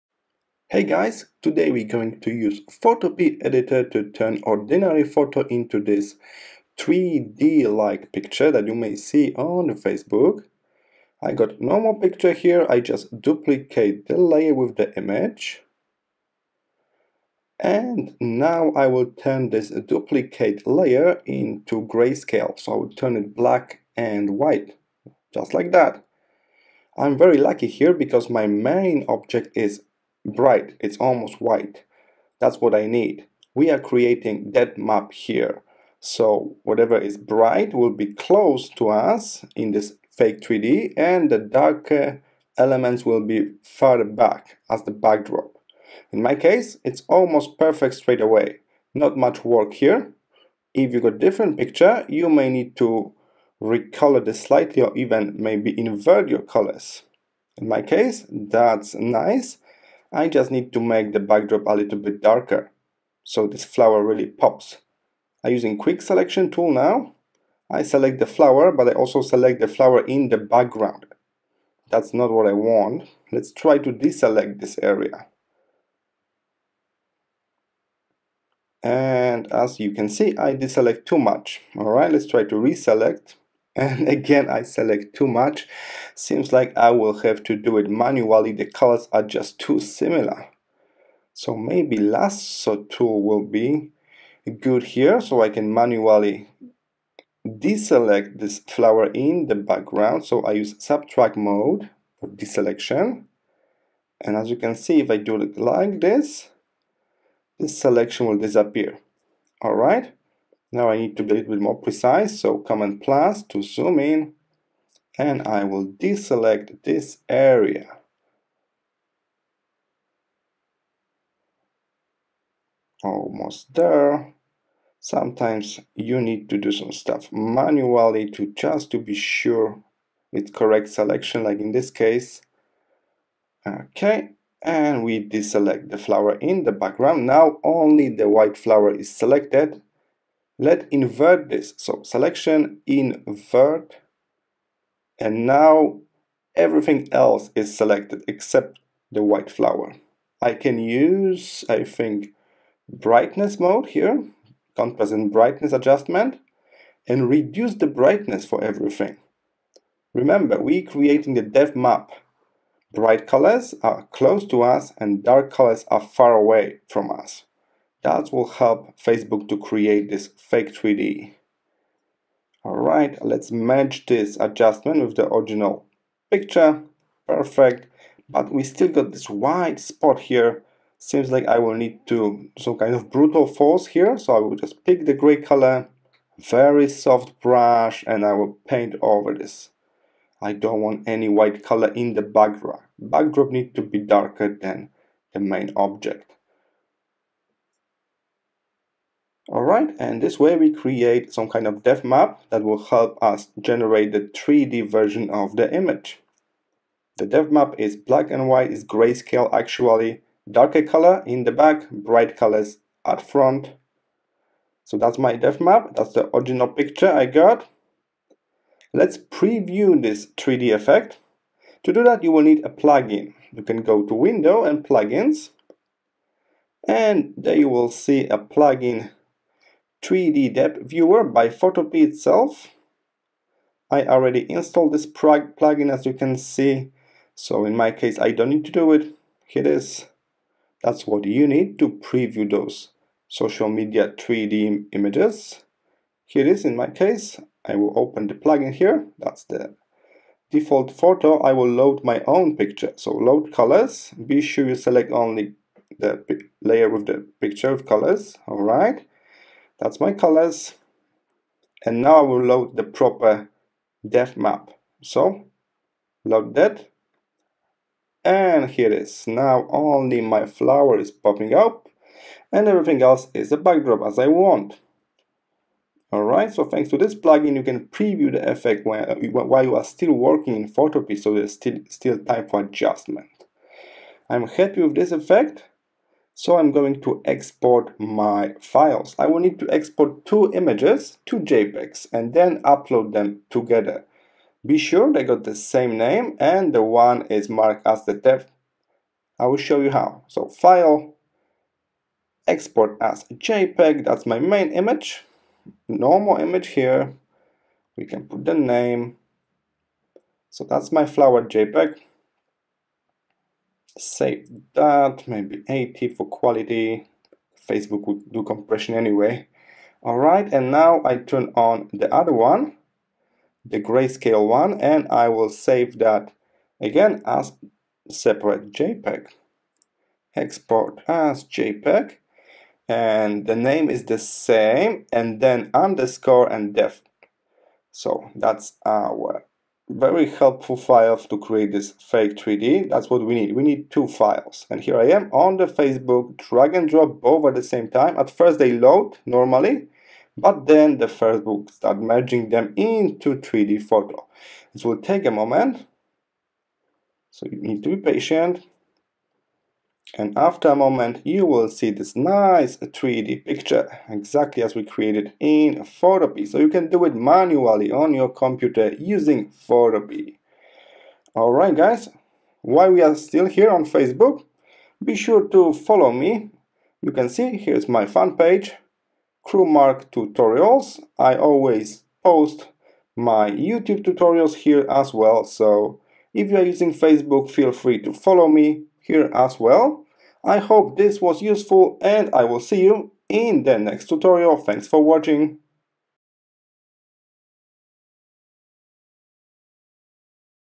How to create a 3D photo for Facebook using Photopea?
You can turn any picture into this fake 3d image using Photopea a free Photoshop alternative. I still don't have my original setup and I have to make this tutorial in iMovie, again sorry for that.